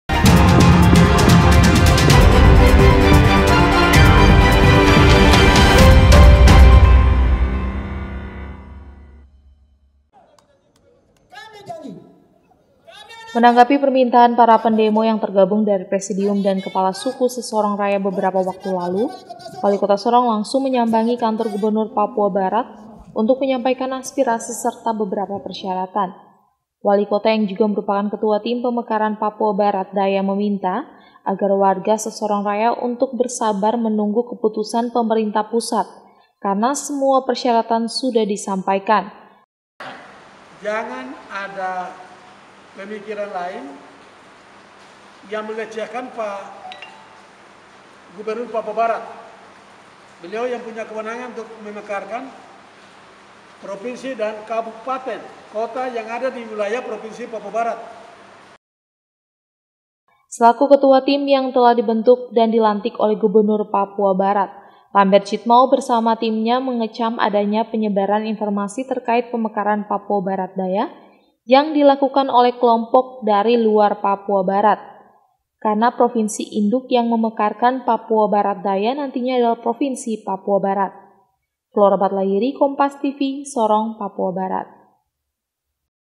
Aspirasi Presidium PBD Sampai Di Tangan Gubernur PB
SORONG, KOMPAS.TV - Menanggapi permintaan para pendemo yang tergabung dari presidium dan kepala suku sesorong raya beberapa waktu lalu, walikota sorong langsung menyambangi kantor gubernur papua barat untuk menyampaikan aspirasi serta beberapa persyaratan.

Walikota yang juga merupakan ketua tim pemekaran papua barat daya meminta warga sesorong raya untuk bersabar menunggu keputusan pemerintah pusat, karena semua persyaratan sudah di sampaikan.

Selaku ketua tim yang telah dibentuk dan dilantik oleh gubernur papua barat, lambert jitmau bersama timnya mengecam adanya penyebaran informasi terkait pemekaran papua barat daya yang dilakukan oleh kelompok dari luar papua barat. Karena provinsi induk yang memekarkan papua barat daya nantinya adalah provinsi papua barat.